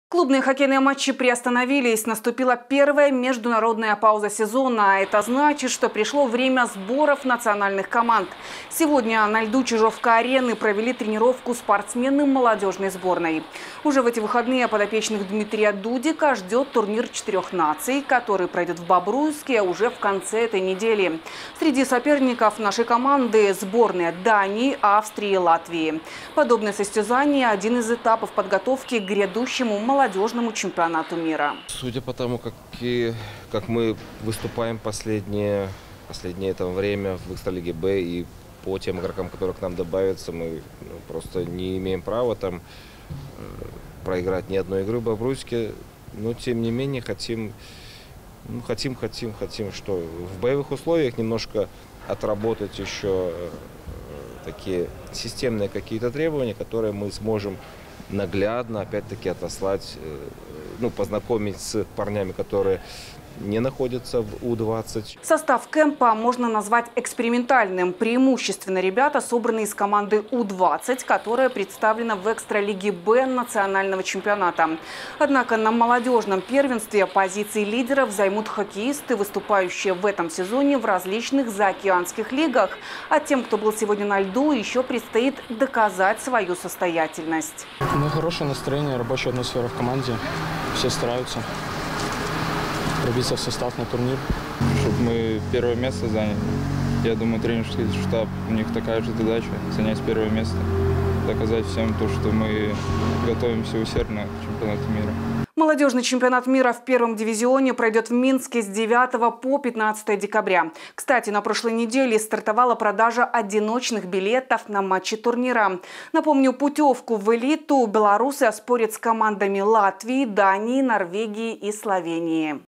Дмитрий Дудик: «Мы просто не имеем права проиграть ни одной игры»
Клубные хоккейные матчи приостановились – наступила первая международная пауза сезона. А это значит, что пришло время сборов национальных команд.
4 ноября на льду «Чижовка-Арены» провели тренировку спортсмены молодежной сборной. Подопечных Дмитрия Дудика ждет Турнир четырех наций, который пройдет в Бобруйске уже в конце этой недели. Среди соперников нашей команды – сборные Дании, Австрии и Латвии.
Подобные состязания – один из этапов подготовки к грядущему молодежного чемпионату мира.